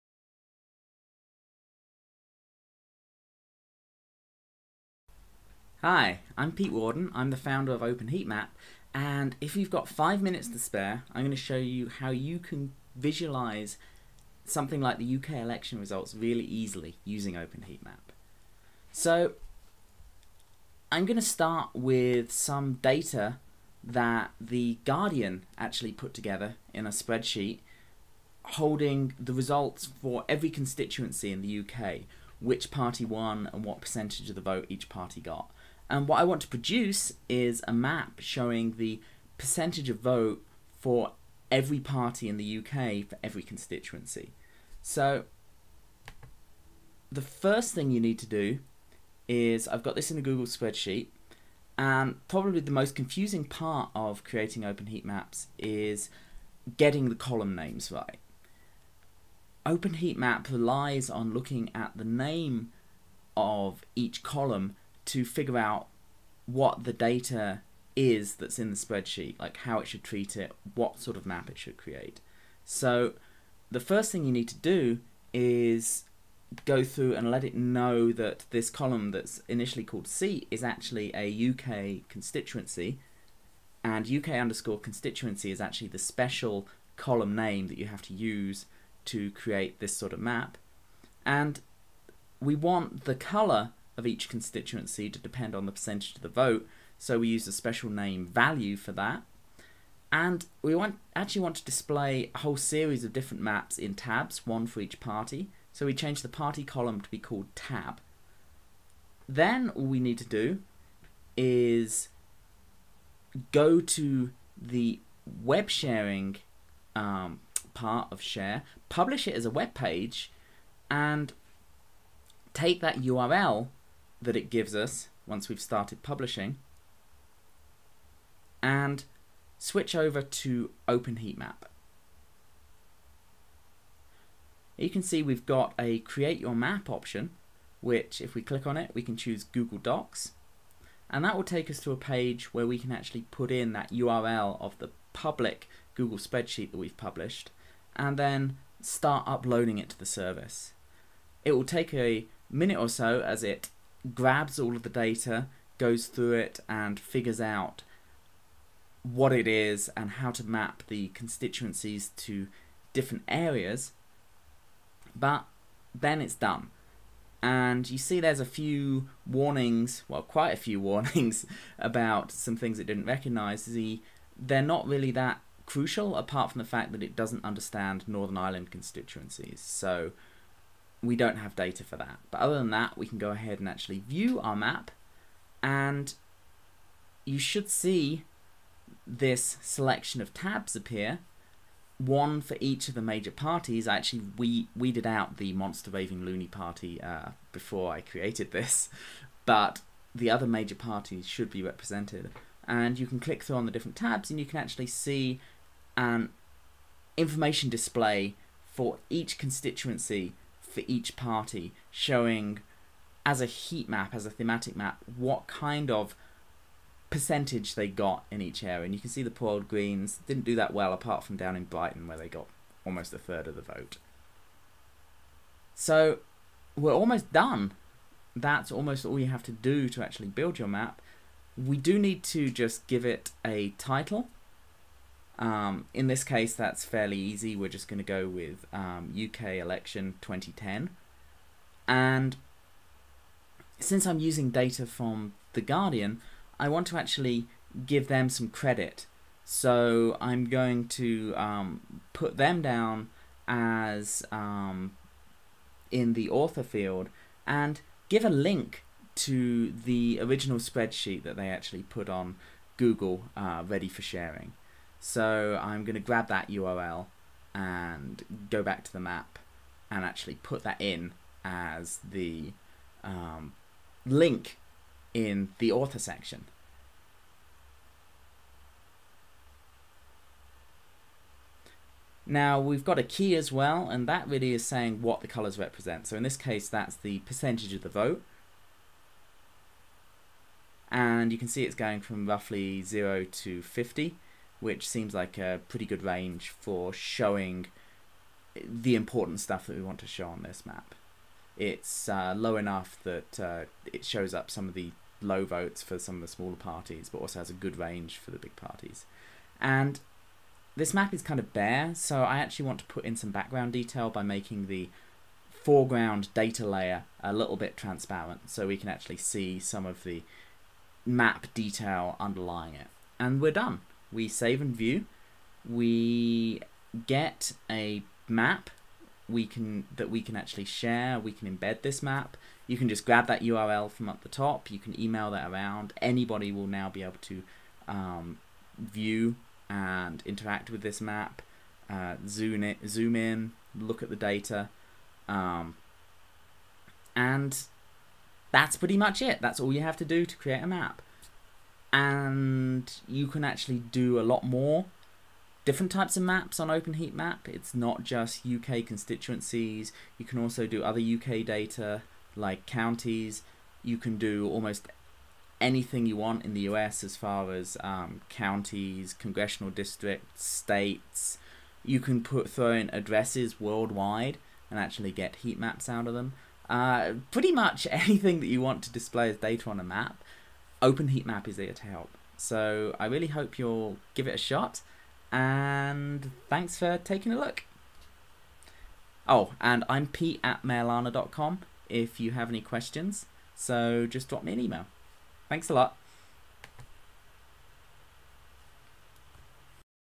How to visualize the UK election results with OpenHeatMap
A quick video guide showing how to create an interactive map from a Google Spreadsheet containing the UK parliamentary election results